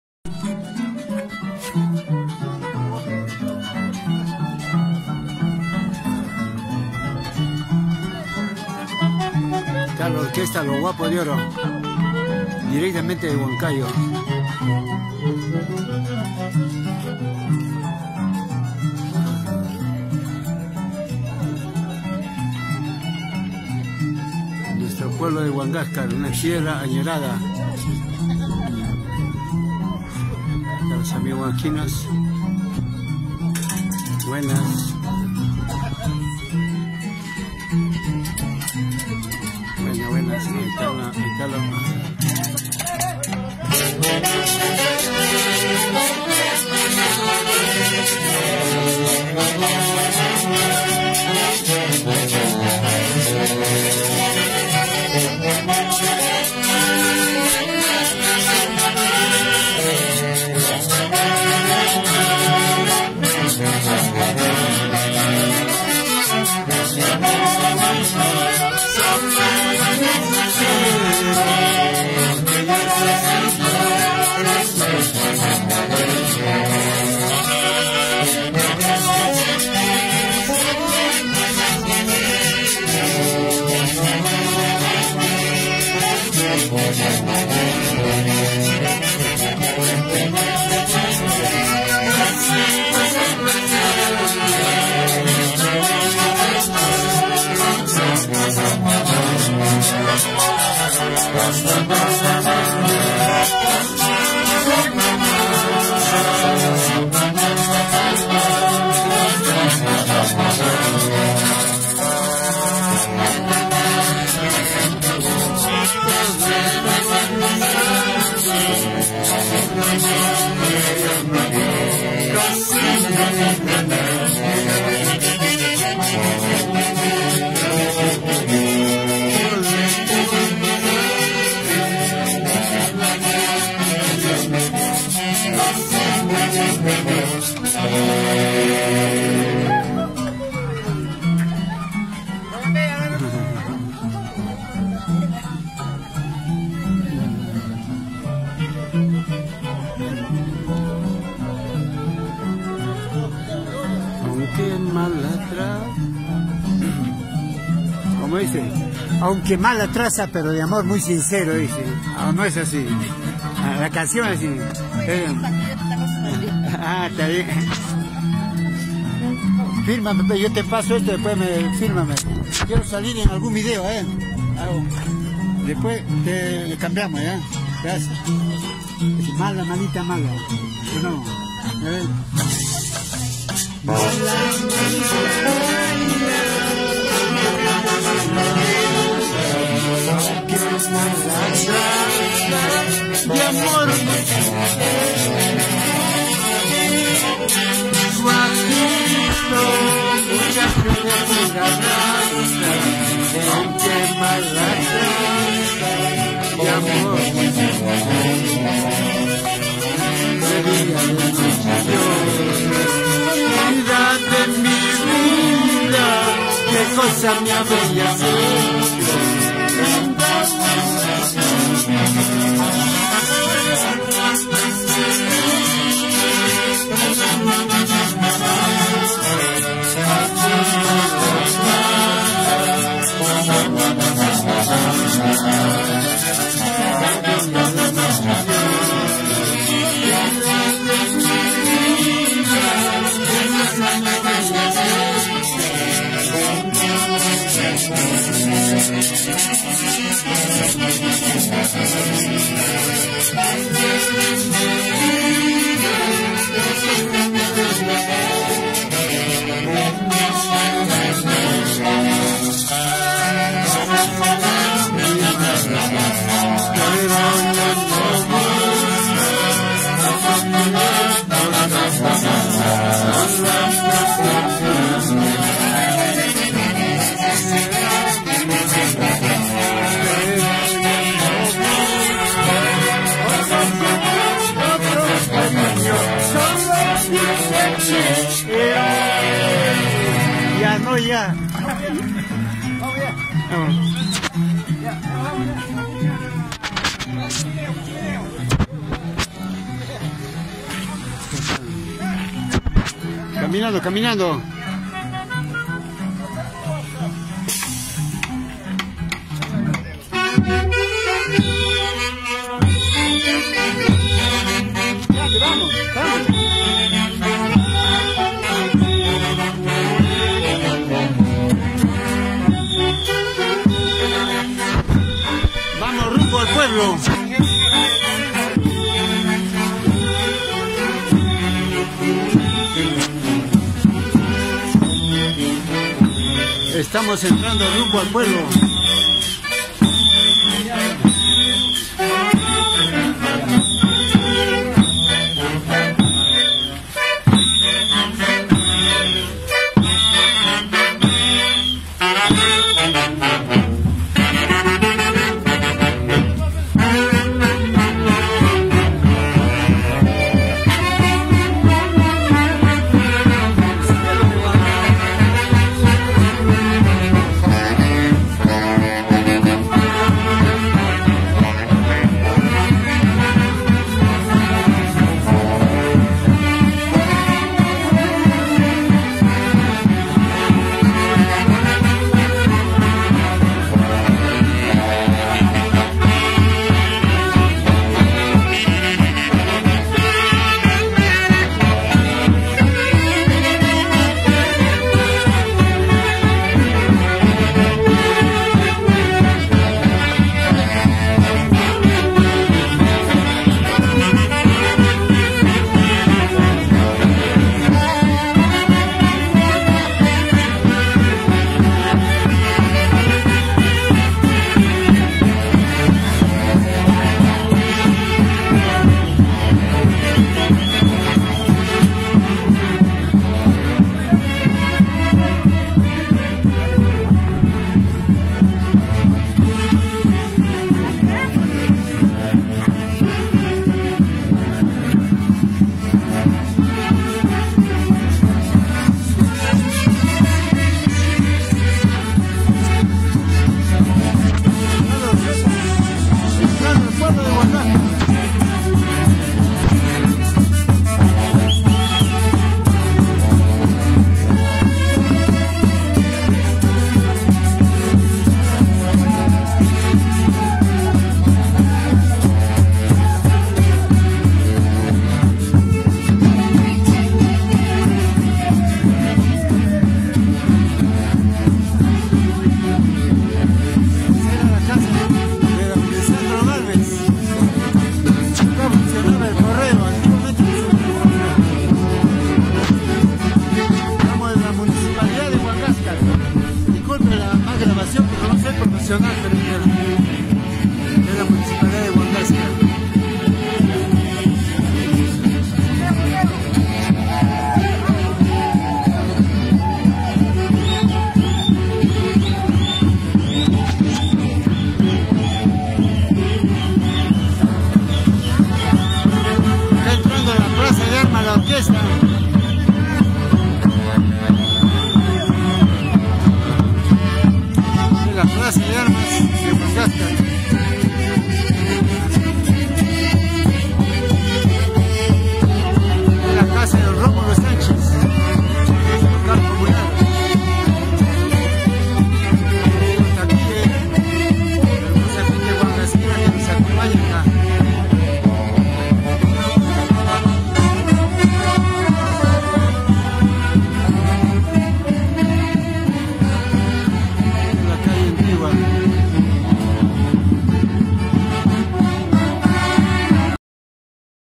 LA TRADICIONAL HERRANZA HUANGASQUINA AÑO 2018.
LA TRADICIONAL HERRANZA HUANGASQUINA AÑO 2018.-Estos hermosos vídeos que lo tengo archivado en mi baúl de los recuerdos pero en mi casa lo comparto con mi madre SABINA PARIONA GUERRA, la señora que trabajaba el año 1953 en el CORREO de Huangáscar donde se comunicaban con la gente del pueblo a través de telegramas. hoy actualmente mi madre tiene 94 años y manda saludos a todo los huangasquinos ESTE VIDEO ES DEL 15 DE agosto del 2018